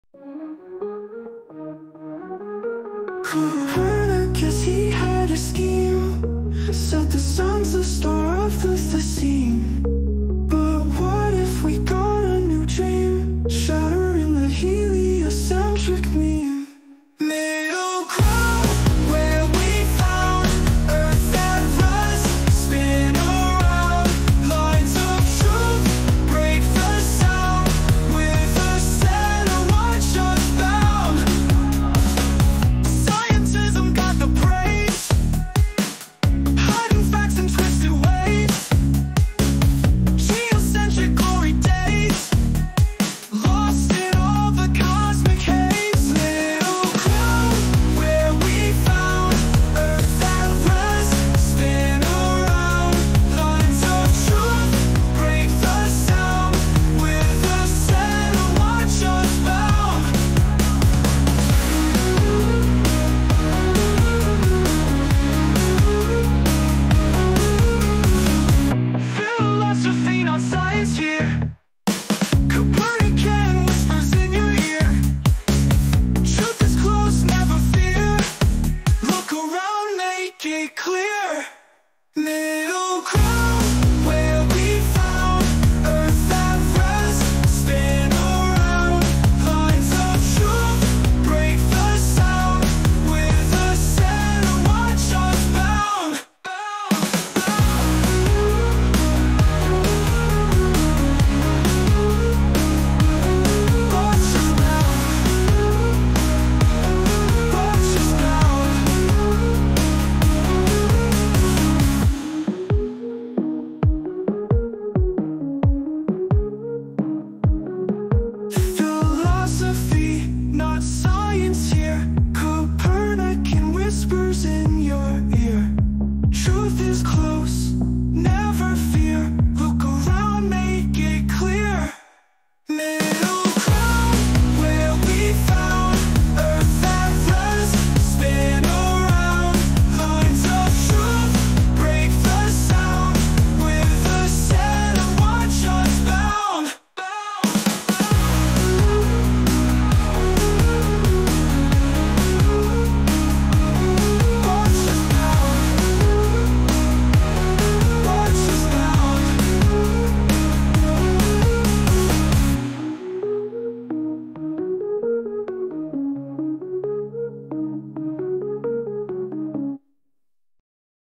Middle Ground Intro Visualizer
Middle Ground is a deep dive into the hidden layers of modern cosmology, questioning the mainstream narratives that have shaped our understanding of the universe. This series uncovers the philosophical biases and overlooked evidence that challenge the conventional views on space, time, and our place in the cosmos. We explore alternative interpretations, including the viability of Geocentrism, to reveal a more complete picture of reality. Join us as we unveil cosmology.